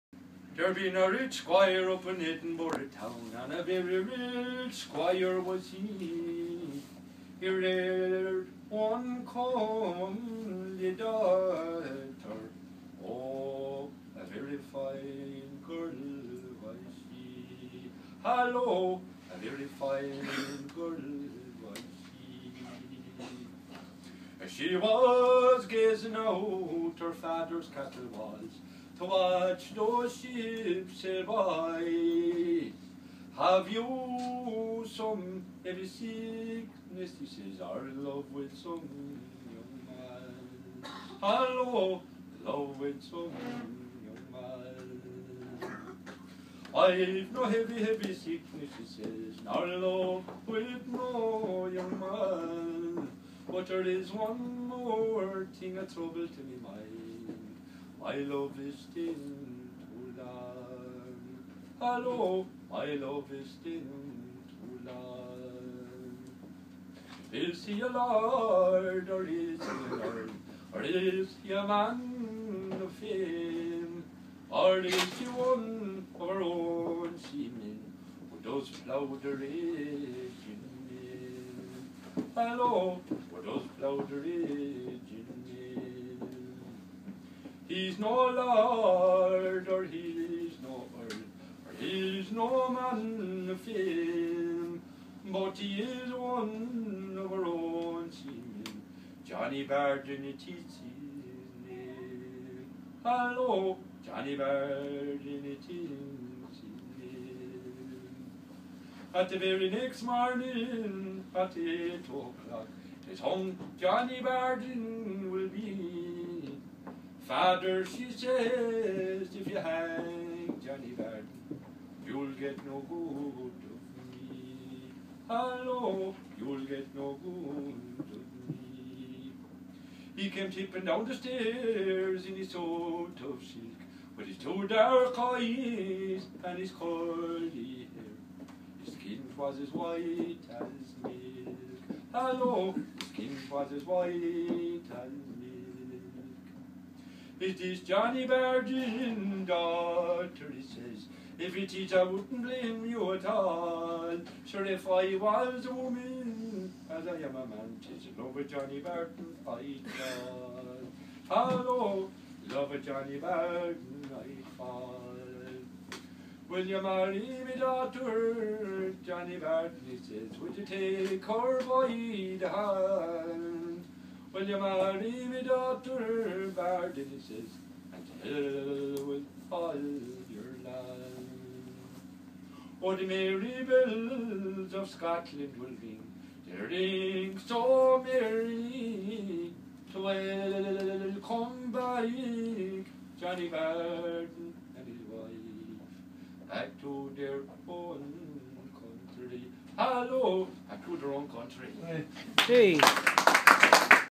Thomas McCarthy. Folk On The Water
Thomas singing on a WW2 minesweeper in Deptford Creek on a chilly March evening